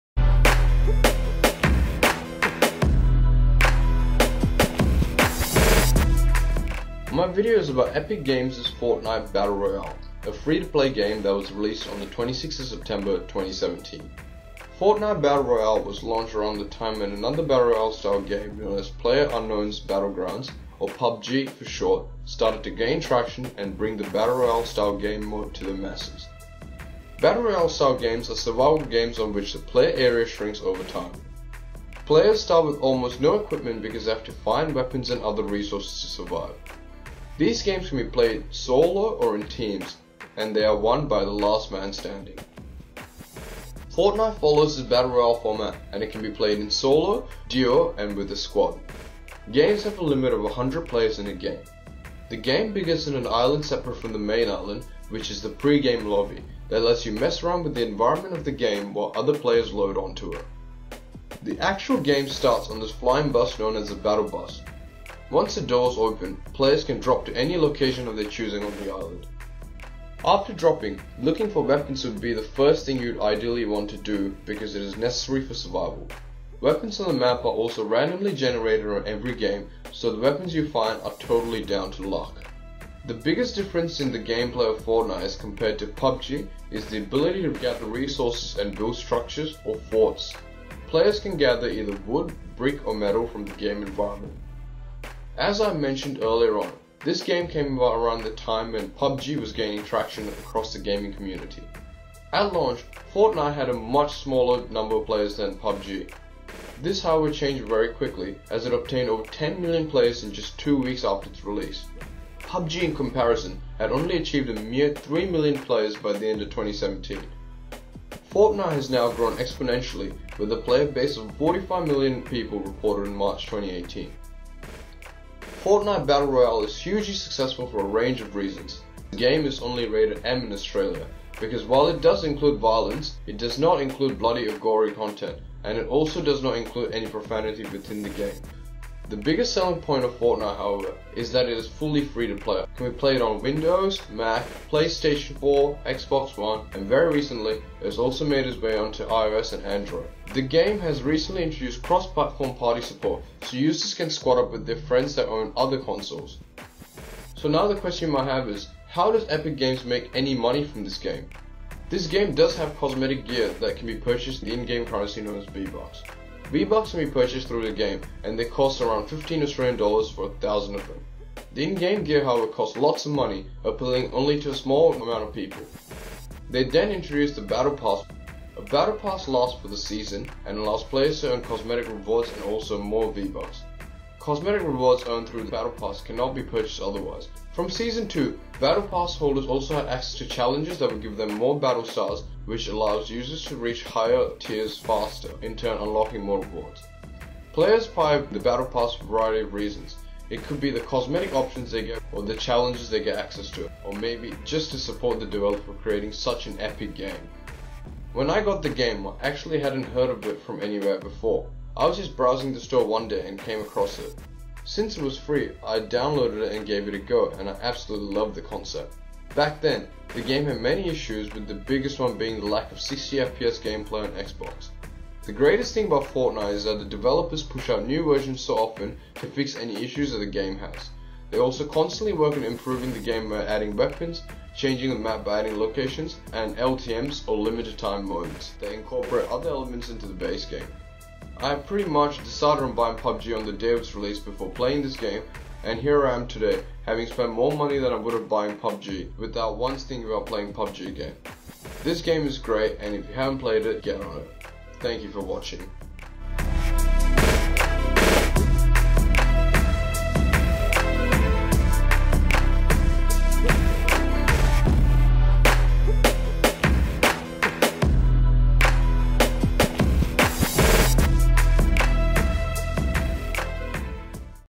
Fortnite Battle Royale Explained
Epic Games' Fortnite: Battle Royale explained in under 5 minutes. This video explores the game in terms of its impact and importance, focusing on the historical, social, economical and technical elements that have made this game so successful. This video is created for NETS3010- Online Games, Play and Gamification.

Information References (ordered alphabetically to adhere to the APA 6th edition referencing guidelines):

Billcliffe, J. (2018). Fortnite: old map vs new map – where is the underground mine and other new locations?

Epic. (2018a). Battle Pass Season 4.

Epic. (2018b). Get Fortnite.

Fillari, A. (2018). Battle Royale Games Explained: Fortnite, PUBG, And What Could Be The Next Big Hit.

Gmelich, K. (2018). Video-Game Stocks Are Selling Off.

Grant, C. (2017). PUBG creators are unhappy with Fortnite: Battle Royale, considering ‘further action’.

Makuch, E. (2017). Fortnite: Battle Royale Has Hit 10 Million Players In Two Weeks.

Makuch, E. (2018). PUBG Passes 3 Million Concurrent Users On PC.

Microsoft. (2018a). Fortnite - 1000 V-Bucks.

Microsoft. (2018b). Fortnite Battle Royale.

ORCZ. (2018). Fortnite Battle Royale: Battle Pass Rewards Season 2.

PLAYERUNKNOWN. (2017). PLAYERUNKNOWN’S BATTLEGROUNDS.

Sledge, K. (2018). Fortnite Releases Patch Focusing on Bug Fixes.

Team, F. (2018). Fortnite Battle Royale State of Development v4.

Tech, D. (2018). Fortnite cross-platform crossplay guide for PC, PS4, Xbox One, Mac and mobile.